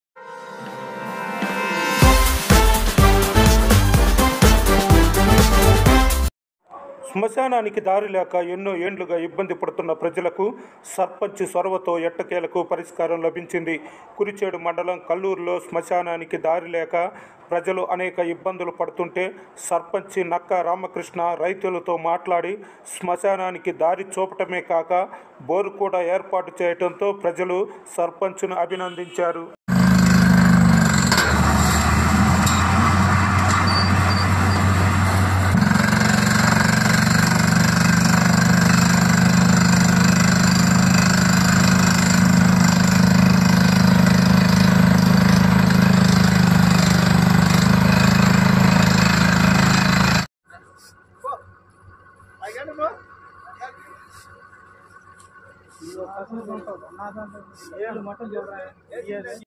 కల్లూరు స్మశానానికి మోక్షం|దారితోపాటు బోరు వేయించిన సర్పంచ్ రామకృష్ణ|AP NEWS 90
NEWS 90#ap news 90#APNEWS90#apnews90#ap news90Telugu#ap news 90 Live#@AP NEWS 90
మండలంలోని కల్లూరు గ్రామం లో ఎన్నో సంవత్సరాల నుండి స్మశానానికి వెళ్లాలంటే దారి లేక గ్రామ ప్రజలు అనేక ఇబ్బందులు ఎదుర్కొనేవారు ఈ సమస్యకు పరిష్కారం చూపాలని ఎమ్మార్వో గారికి ఎన్నోసార్లు అర్జీలు ఇచ్చిన పరిష్కారం దొరకలేదు గ్రామ ప్రజలు అధికారులు చుట్టూ తిరిగిన ప్రయోజనం లేకుండా పోయింది గ్రామ సర్పంచి నక్క రామకృష్ణ పొలాల రైతులతో మాట్లాడి ఒప్పించి స్మశానానికి దారి ఏర్పాటు చేశాడు ఎన్నో సంవత్సరాల నుండి పరిష్కారం దొరకని సమస్యకు గ్రామ సర్పంచి నక్క రామకృష్ణ పరిష్కారం చూపించాడని గ్రామ ప్రజలు ఆనందం వ్యక్తం చేశారు దారి వేయించటం తో పాటు స్మశానంలో బోరు కూడా వేయించటం జరుగుతుంది ఈ కార్యక్రమంలో మాజీ డిసి చైర్మన్ నక్క  ఎర్రబెల్లి కొండయ్య గ్రామ పెద్దలు పాల్గొన్నారు
Santhosh Kumar kola
D-N-5-135 besid Sri Raghavendra high school